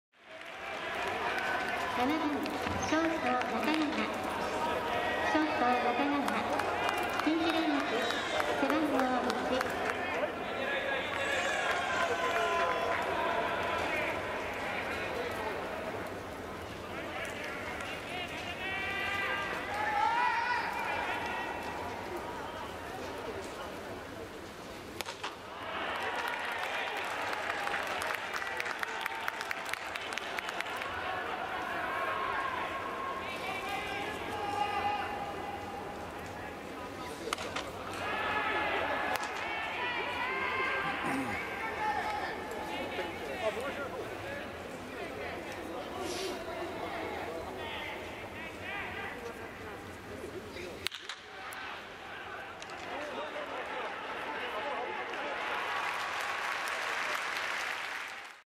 セガサミー 中川 智裕 選手（近大付高→近畿大学）：第91回都市対抗野球大会
2021年11月22日に東京ドームで開催された第91回都市対抗野球大会・1回戦 セガサミー（東京都）vs トヨタ自動車（トヨタ）でのセガサミーのルーキー・中川智裕選手の打撃シーン。